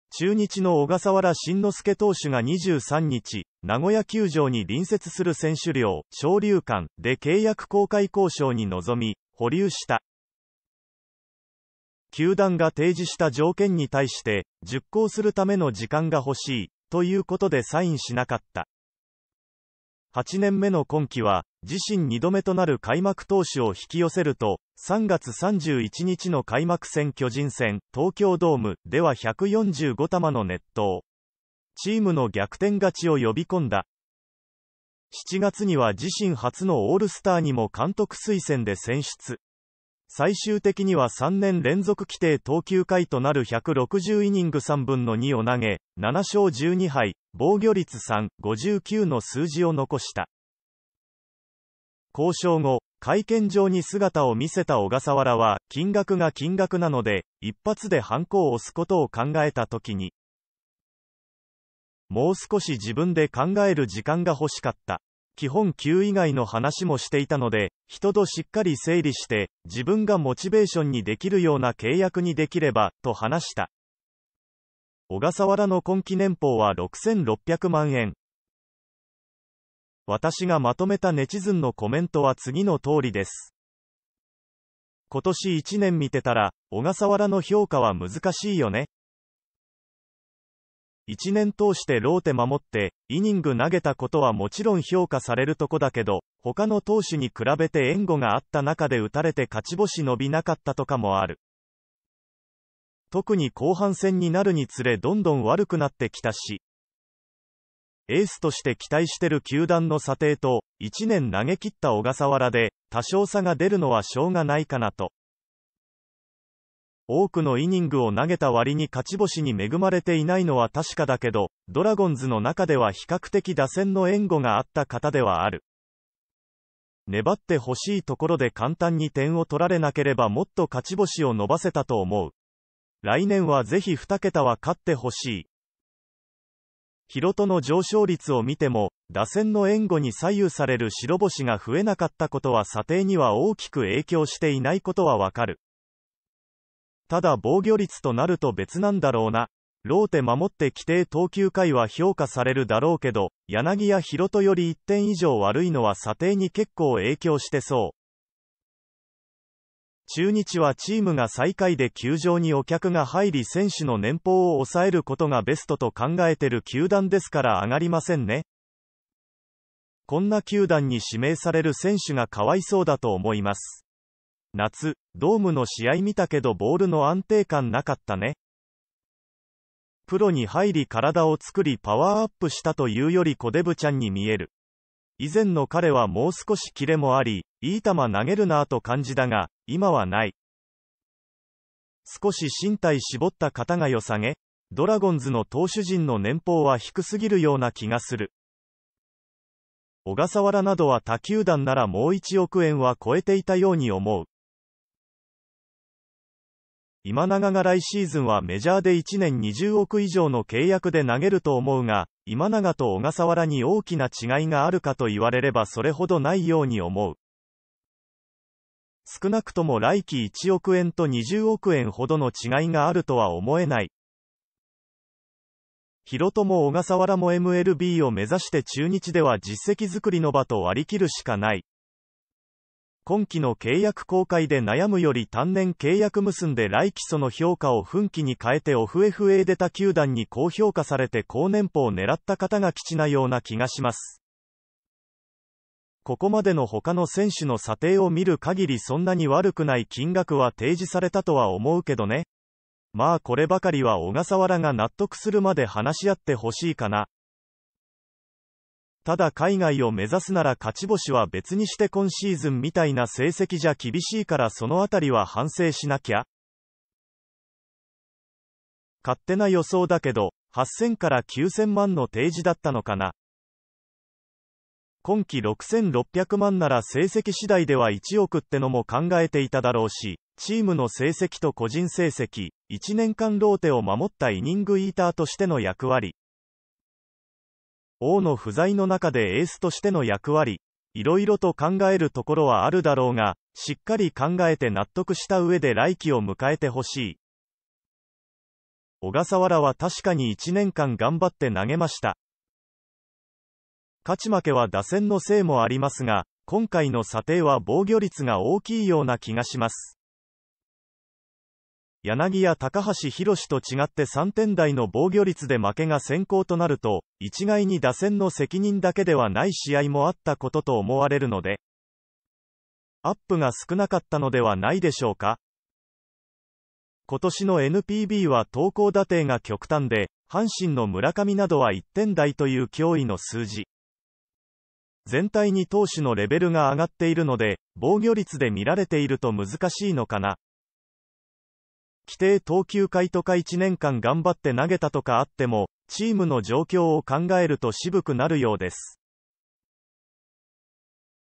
小笠原慎之介が保留、今オフの契約更改チームで初めて「金額が金額なので」「もう少し自分で考える時間がほしかった」
８年目の今季は、自身２度目となる開幕投手を引き寄せると、３月３１日の開幕戦巨人戦（東京ドーム）では１４５球の熱投。